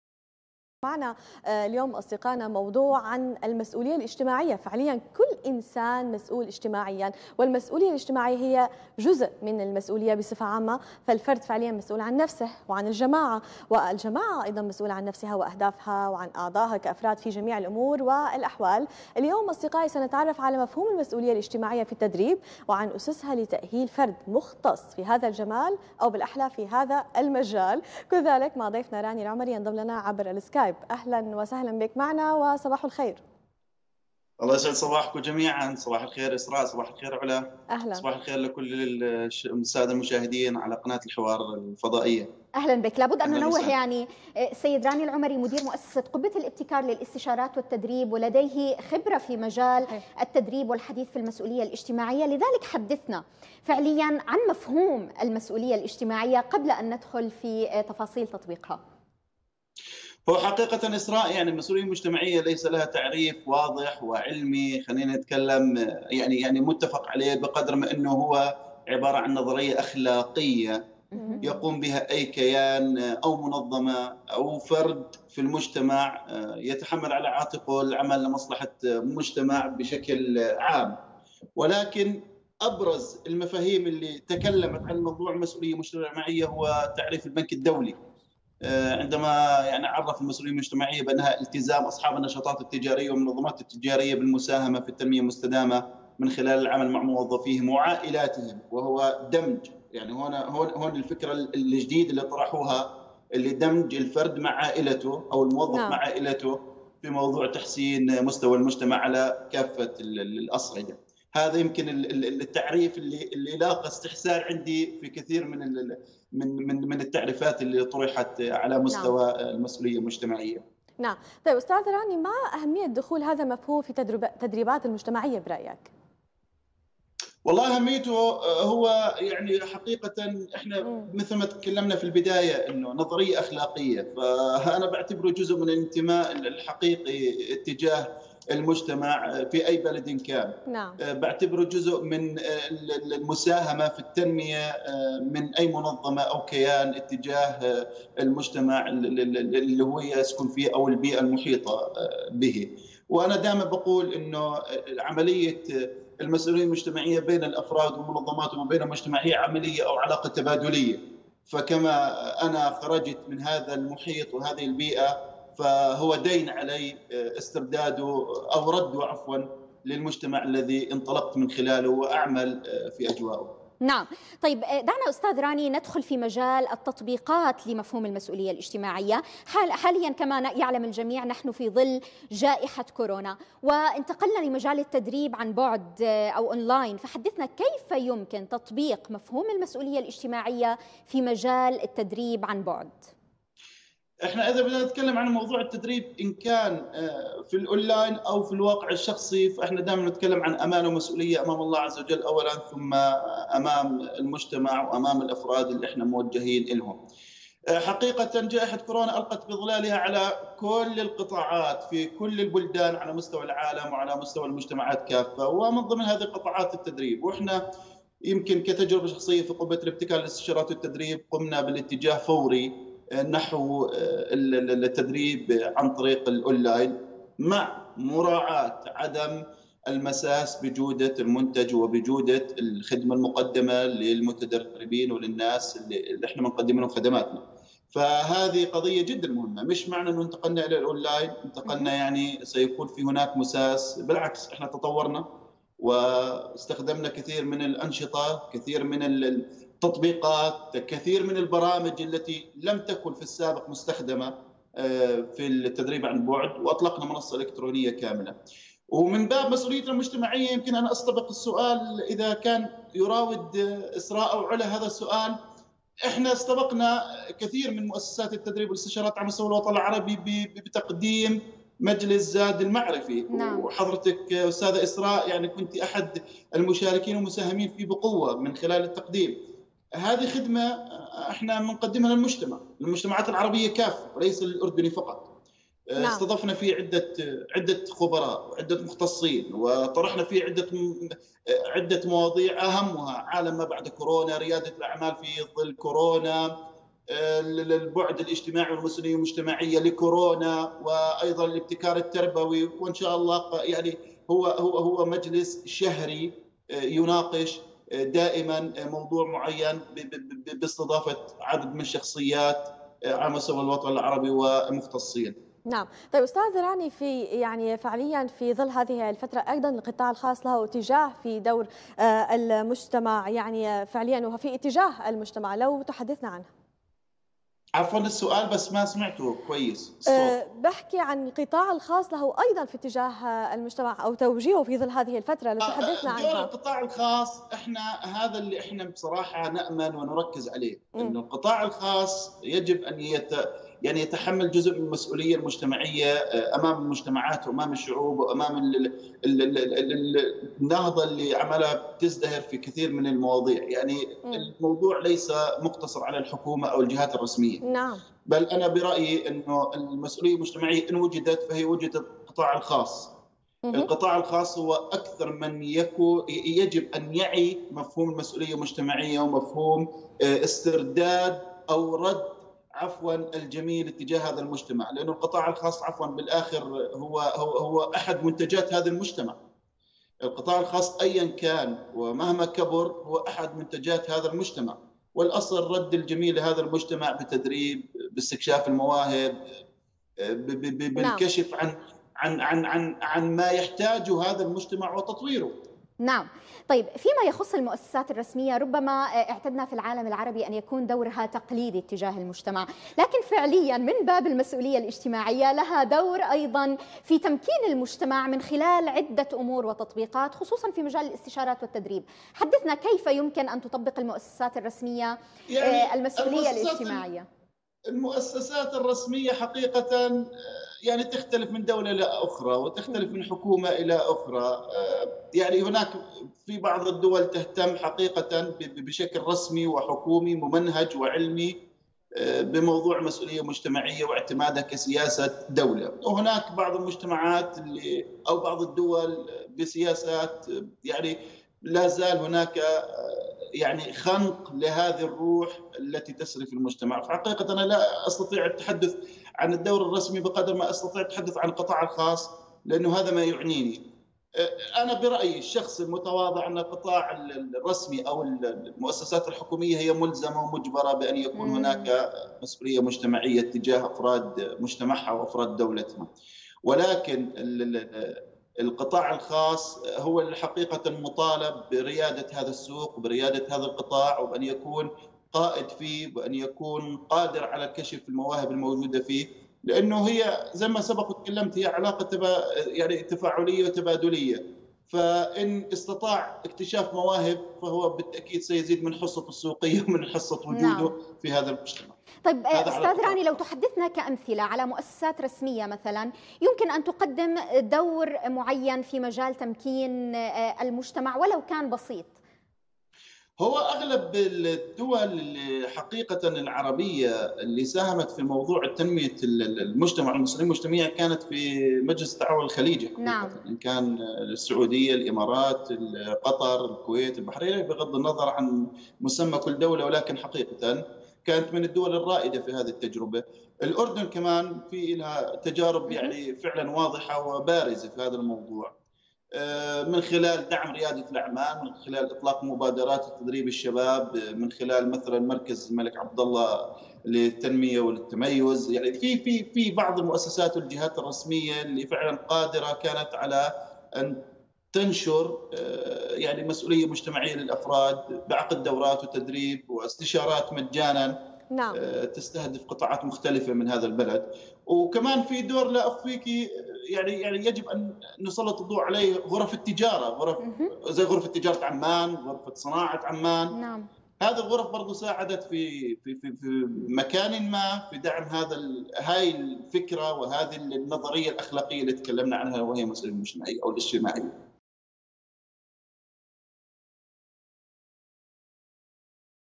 كيف تتم عملية تهيئة الفرد لتحمل "المسؤولية الاجتماعية"؟
كيف تتم عملية تهيئة الفرد لتحمل "المسؤولية الاجتماعية"؟.. المدير العام لقبة الابتكار والاستشارات والتدريب راني العمري يقدم الإجابة